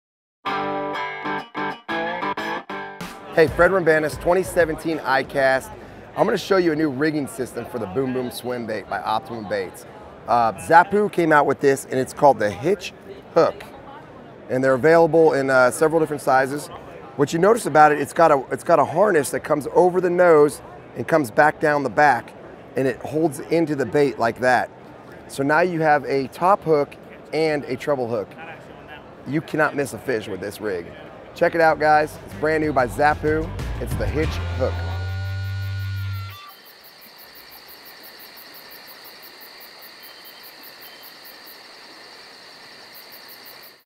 Zappu Hitch Trailer Hook with Fred Roumbanis | ICAST 2017
Tackle Warehouse discusses the new Zappu Hitch Trailer Hook with Fred Roumbanis at the 2017 ICAST Show in Orlando.